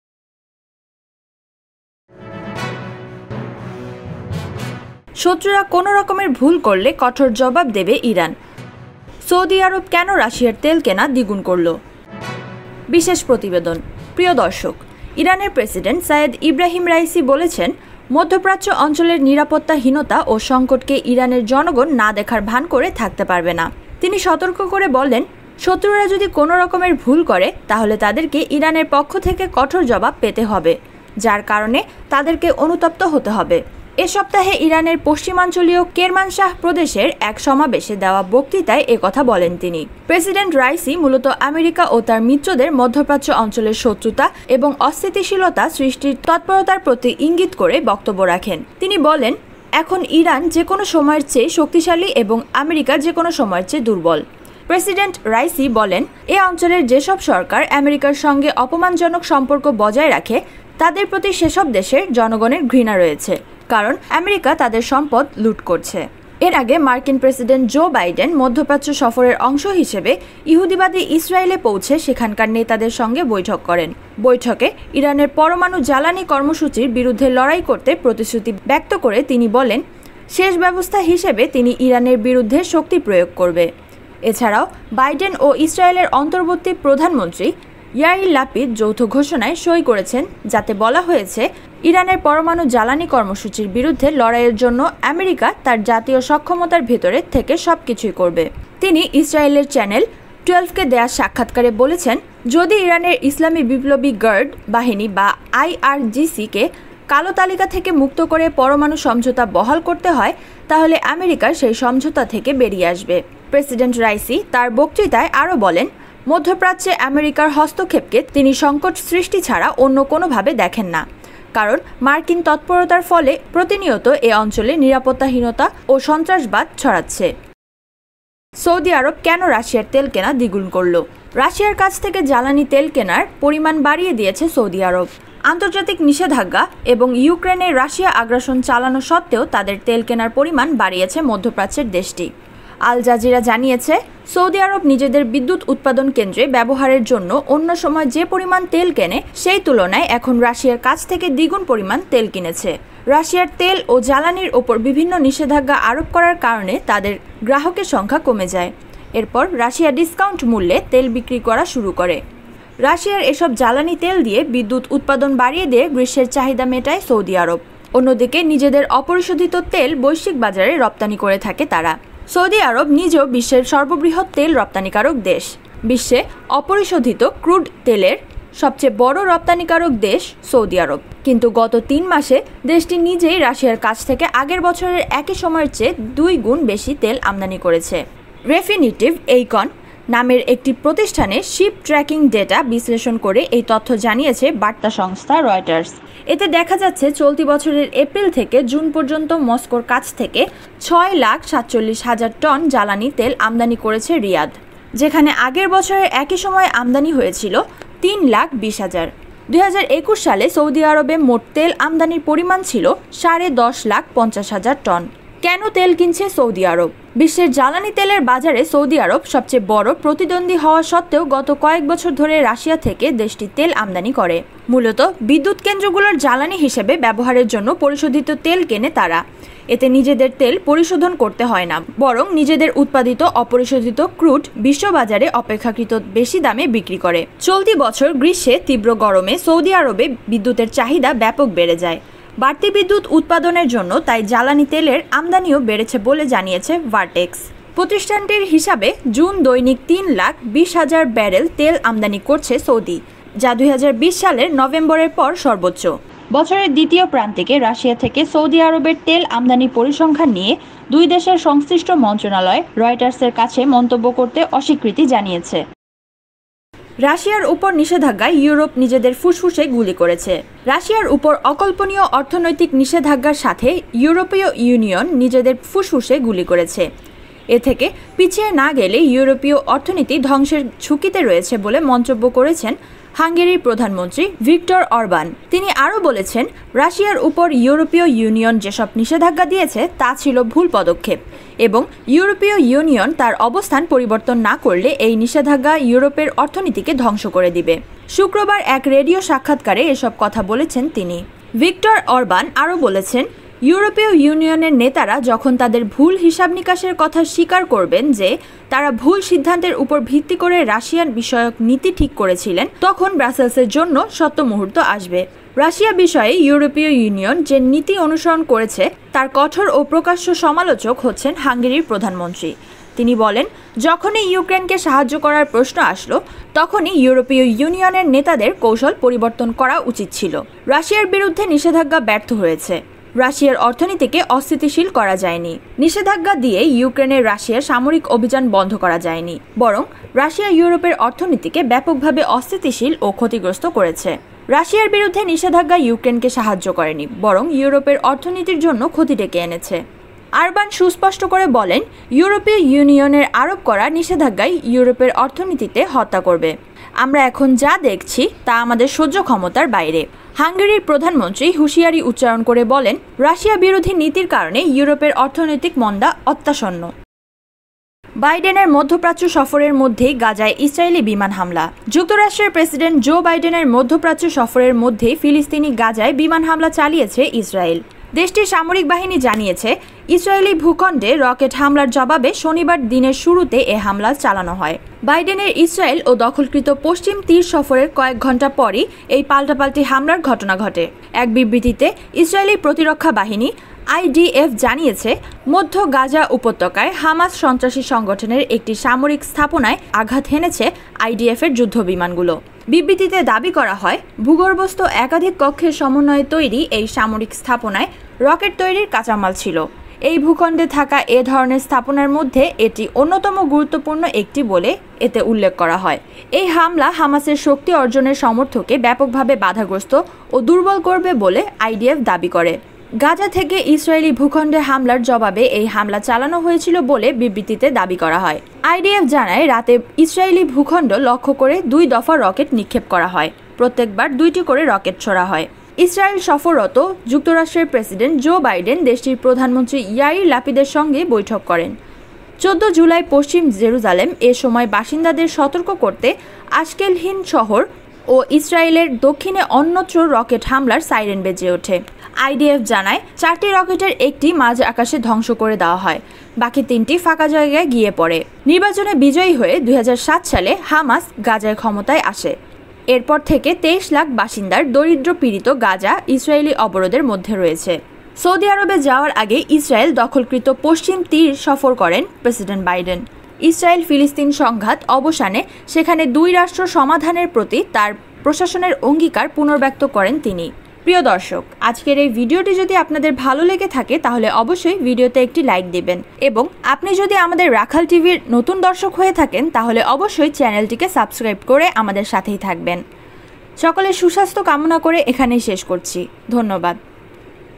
‘শত্রুরা কোনো রকমের ভুল করলে কঠোর জবাব দিবে ইরান’ সৌদি আরব কেন রাশিয়ার তেল কেনা দ্বিগুণ করলো?
আরো দেখুন.

Dear Sir/Madam!..